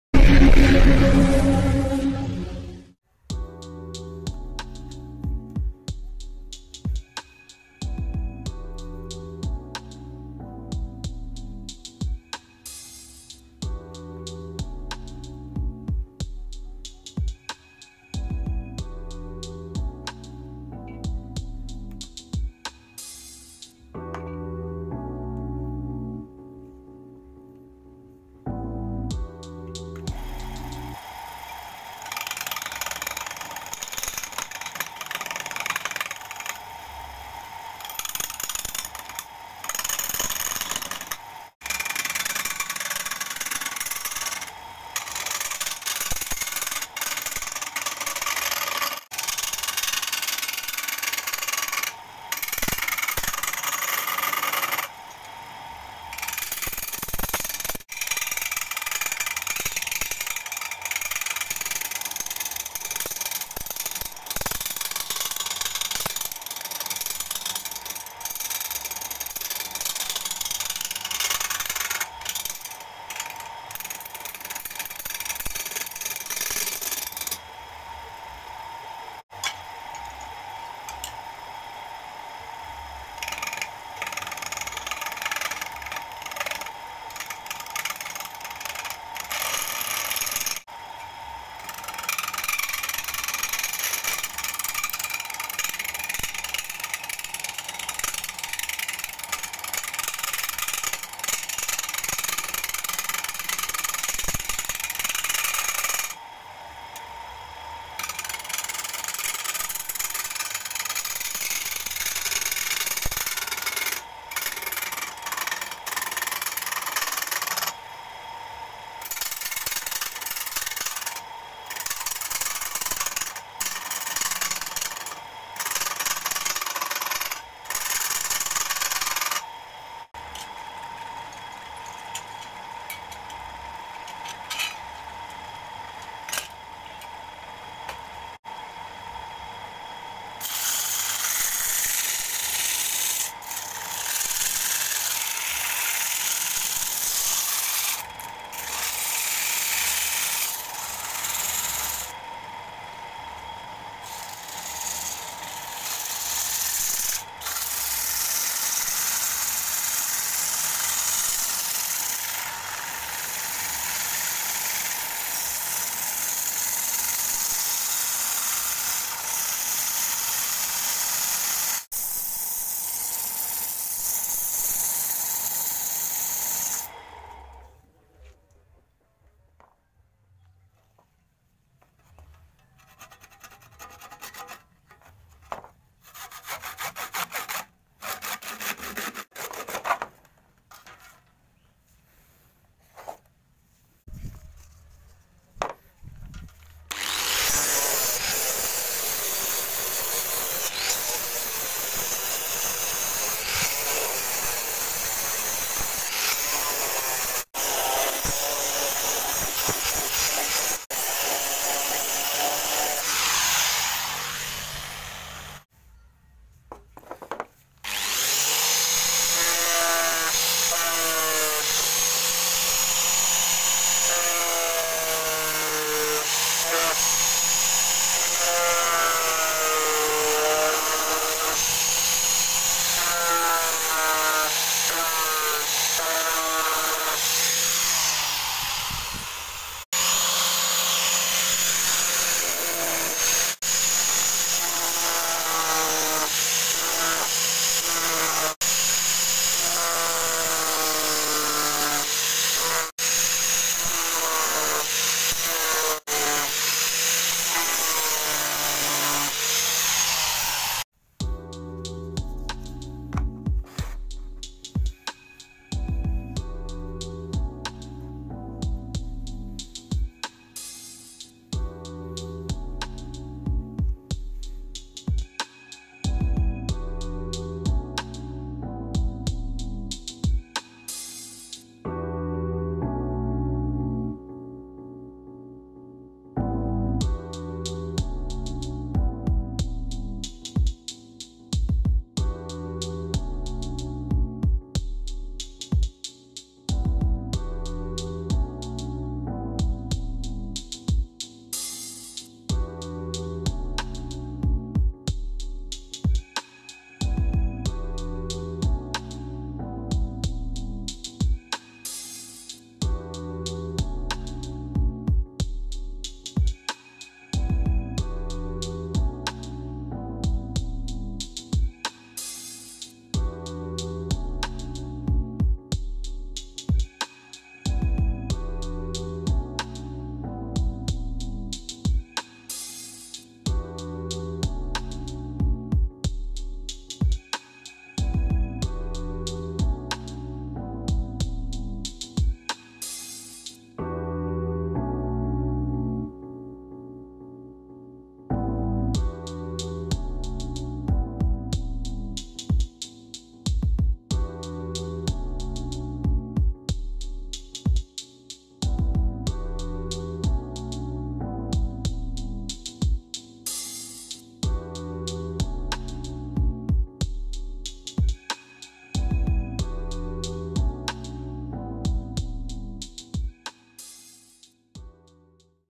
Keunikan Batok Kelapa Yang Mengandung Pita Emas... bubut kayu
membuat kerajinan batok kelapa